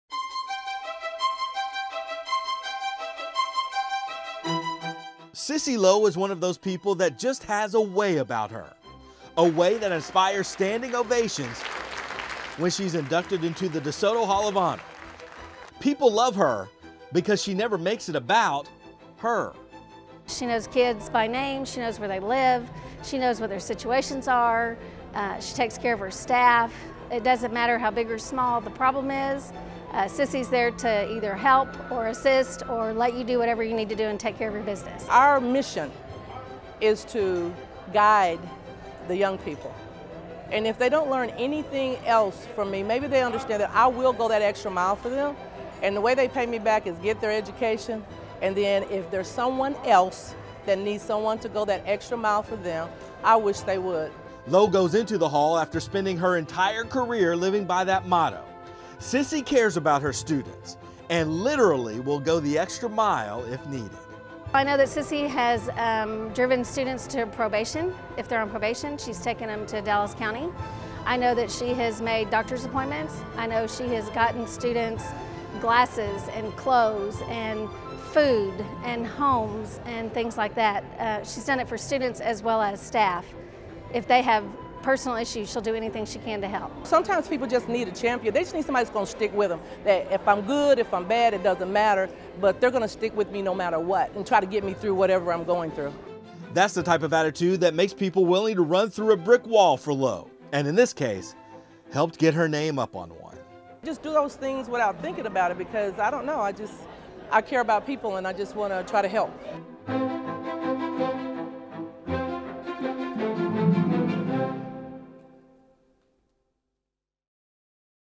Sissy Lowe Enters the DeSoto ISD Hall of Honor
Melvlyn "Sissy" Lowe has been in education for more than three decades.  She is the first African-American principal in DeSoto ISD.  She has won the TMEA Distinguished Educator Award, TAASP Excellence Award, Principal of the Year, and many more.